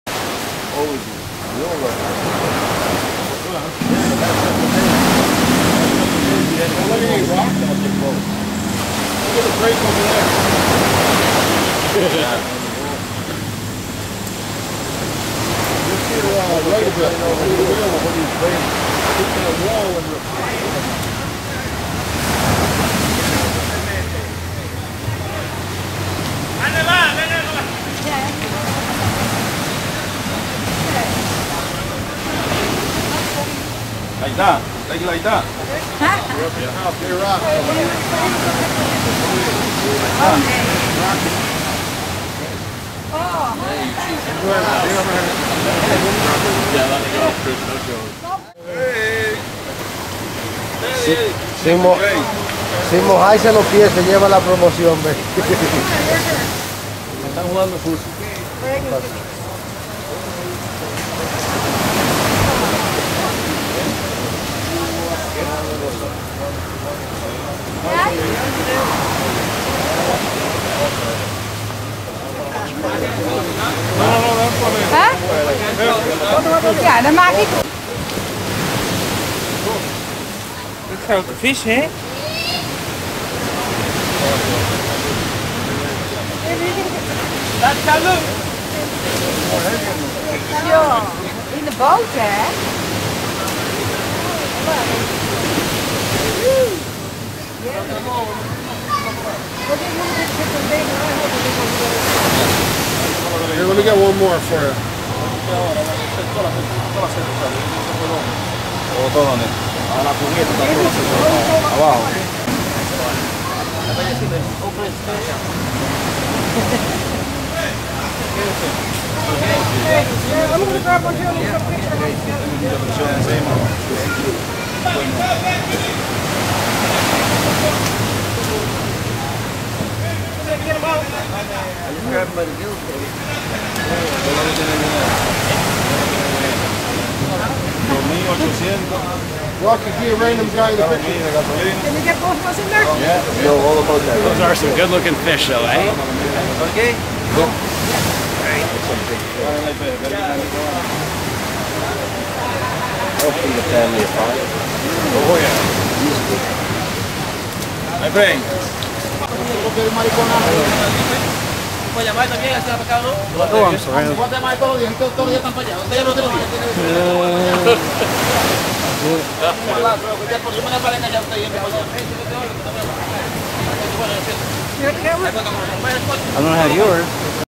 Dominican Fishing trip Nov 2011
After the trip looking at the catch ect